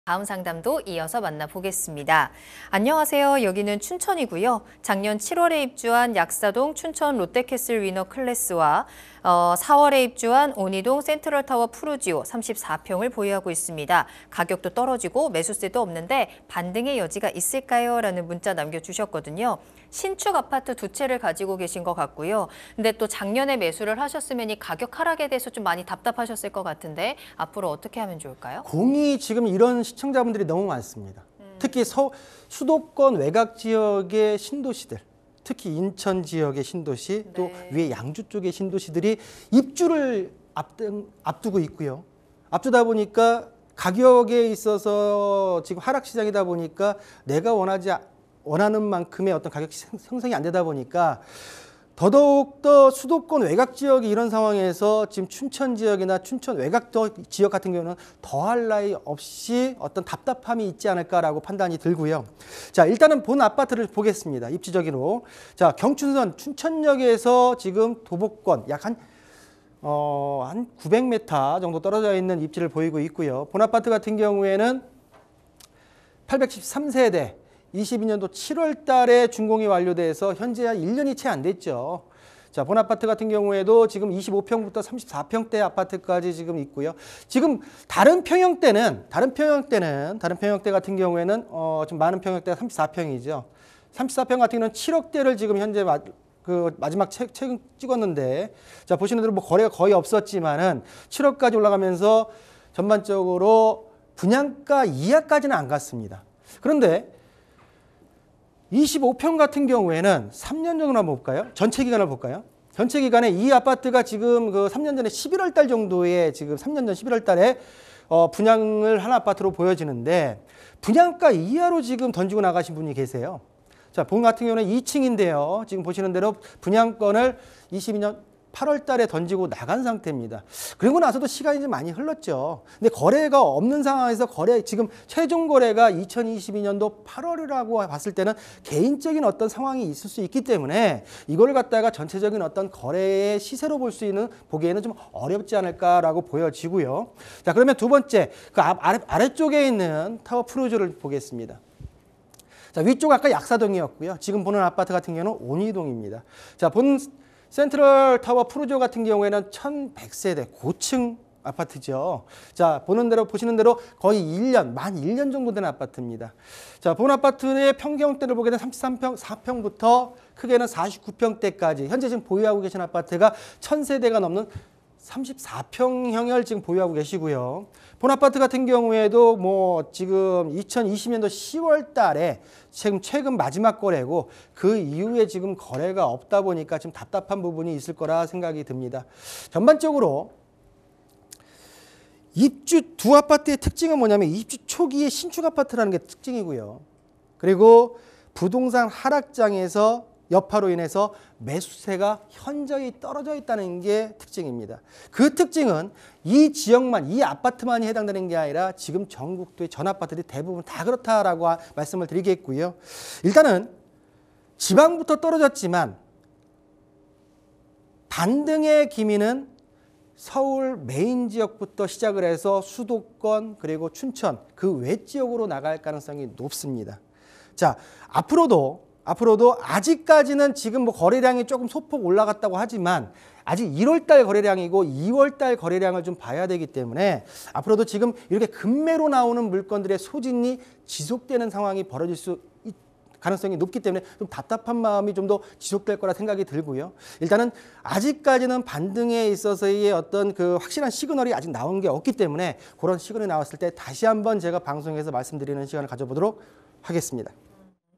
부동산시그널 : 춘천시 약사동, 온의동 아파트 두채 있는데... 반등 여지 있을까?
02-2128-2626으로 문의 바랍니다!

박성규 대표(JYP홀딩스)의 다섯 번째 상담

안녕하세요 여기는 춘천이고요
작년 7월 입주한 약사동 춘천롯데캐슬위너클래스 25평과
4월 입주한 온의동 센트럴타워푸르지오 34평 보유 중입니다
가격도 떨어지고 매수세도 없는데 반등 여지 있을까요?

-Btv CH158
-U+tv CH170
-D'LiVE CH188